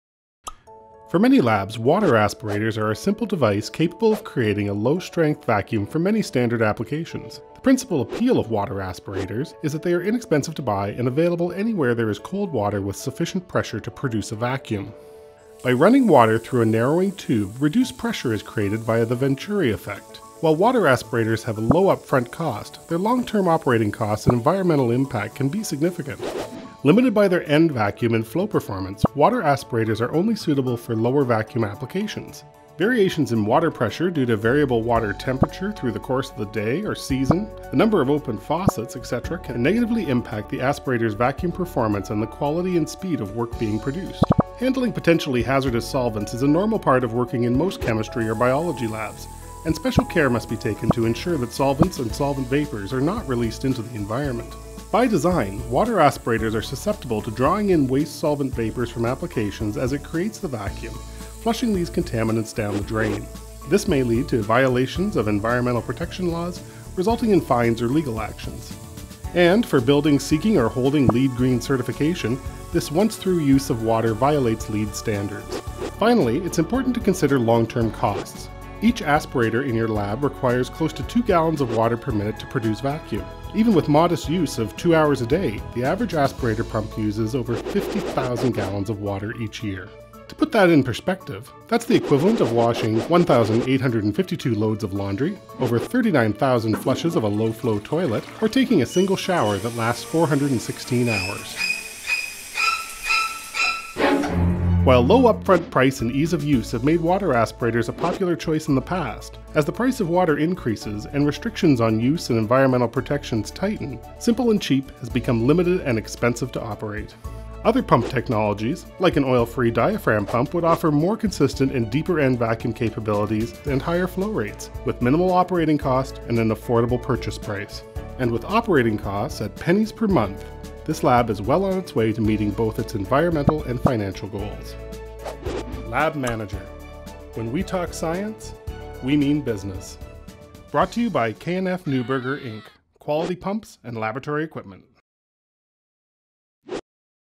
Time to Replace Your Water Aspirator
KNF oil-free, laboratory vacuum pumps provide a lower cost of ownership, and much less of an environmental impact than water aspirators.